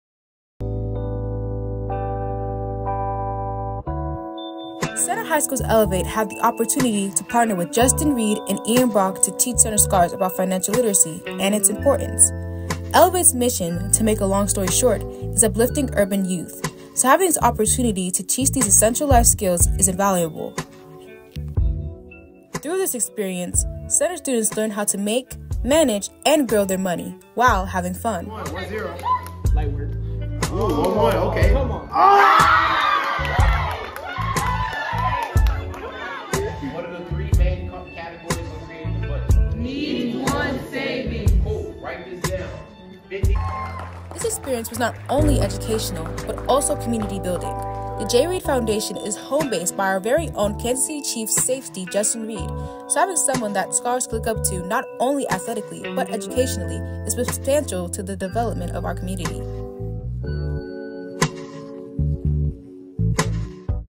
Elevate- Financial Literacy (w/Justin Reid)
The Elevate program at CHS brought in KC Chiefs Safety Justin Reid and experts to explain the value of financial literacy.